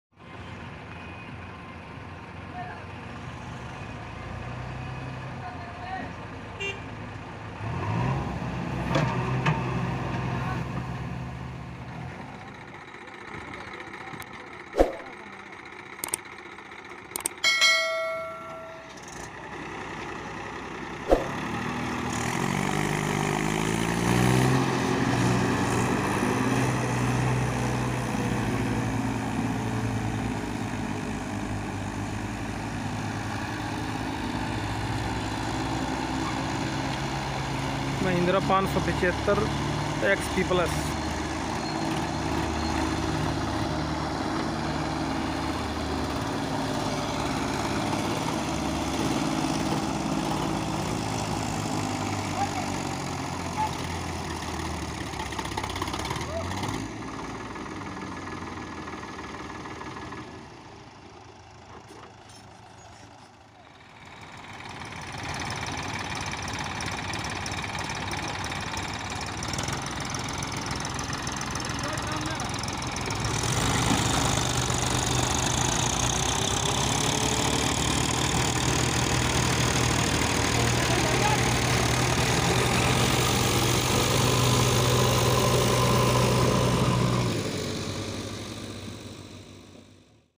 Mahindra 575 DI xp plas pulling full loaded trolla | महिंद्रा 575 को JCB की help से निकालना पड़ा
Mahindra 575 xp plas pulling full loaded trolla | महिंद्रा 575 को JCB की help से निकालना पड़ा